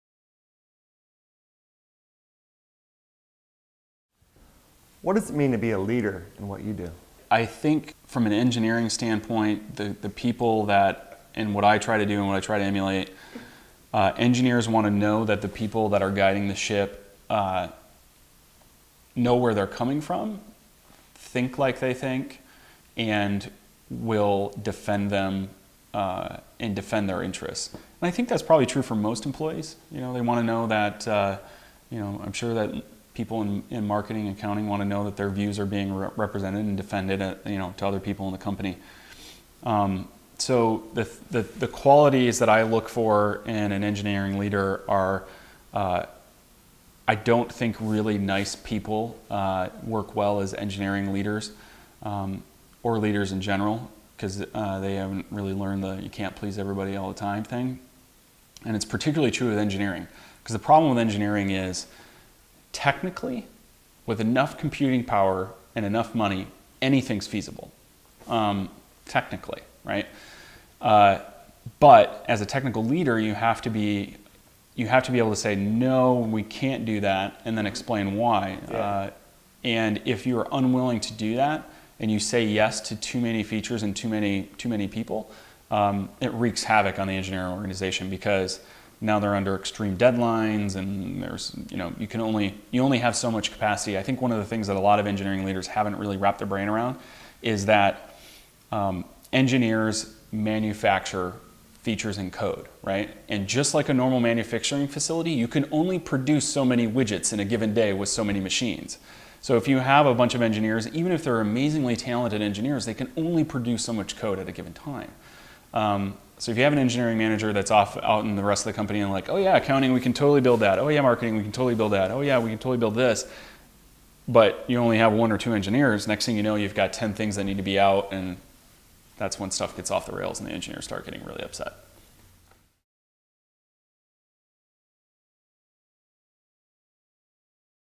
How to Lead an Engineering Team - Joe Stump
In Chapter 12 of 17 in his 2011 Capture Your Flag interview with host Erik Michielsen, Internet entrepreneur and SimpleGeo CTO Joe Stump notes what it means to be an engineering leader. Motivating and managing technical talent requires many things. Engineers want leaders to understand their responsibilities and interests. Stump believes engineering leaders must accept they will be unable to please everyone and then follow through making difficult decisions to keep product goals aligned with available resources and timeframes. Stump is the co-founder and CTO at SimpleGeo a San Francisco-based mobile location infrastructure services company. Previously Stump was Lead Architect at Digg. He programs in PHP, Python, Django and enjoys scaling websites. He earned a BBA in Computer Information Systems from Eastern Michigan University.